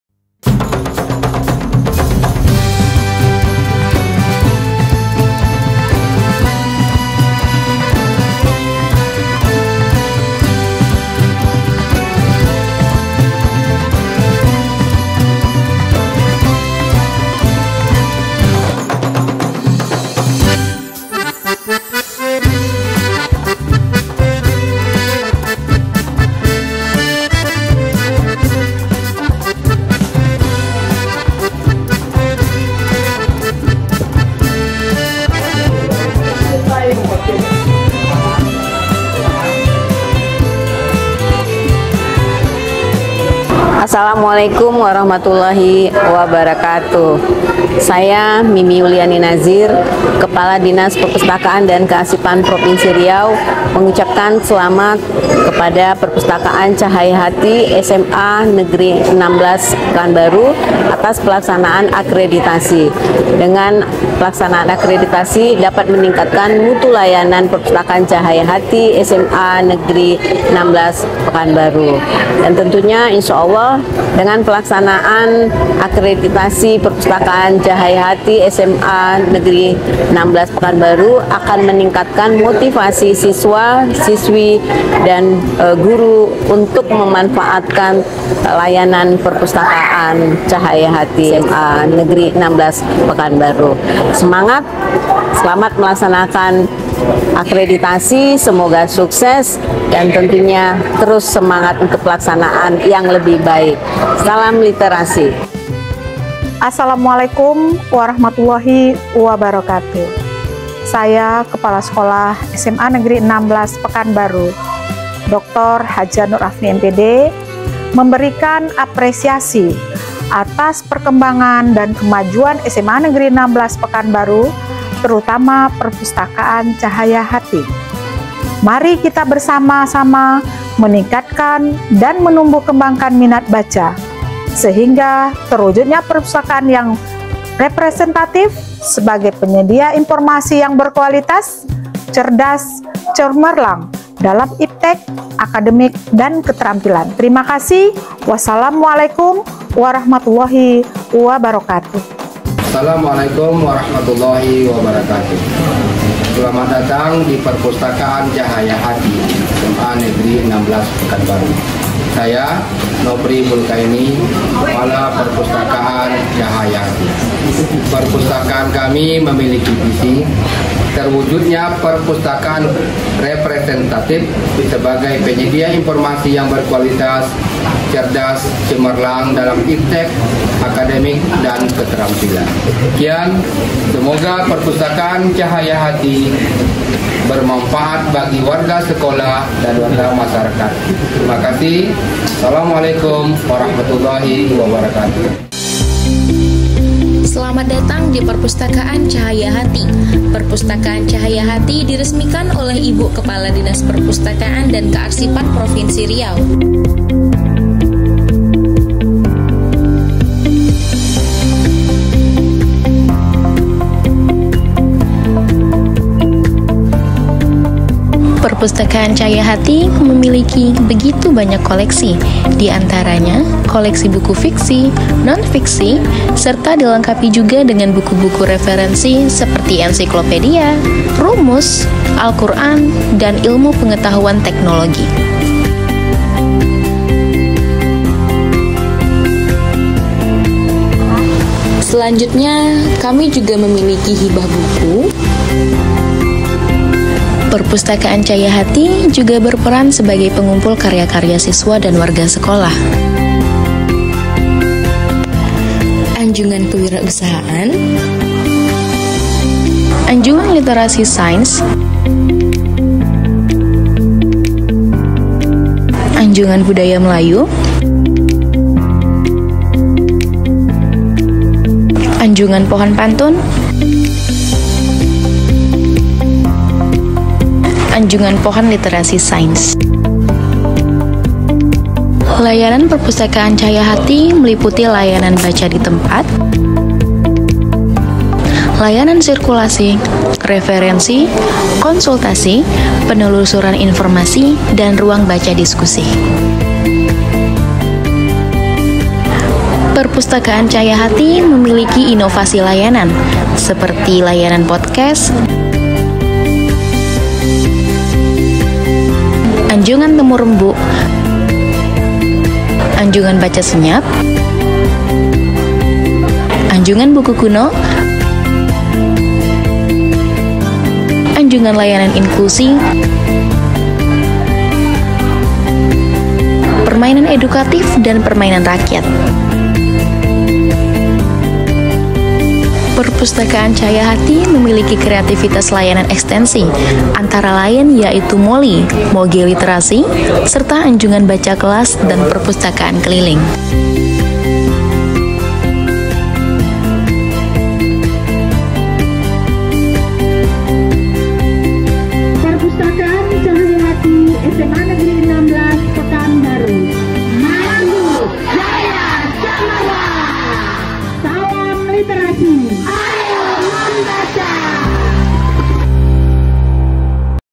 PROFIL PERPUSTAKAAN CAHAYA HATI SMAN 16 PEKANBARU
Perpustakaan sekolah adalah sarana yang menyediakan informasi, ilmu pengetahuan, dan kebudayaan untuk menunjang kegiatan belajar mengajar di sekolah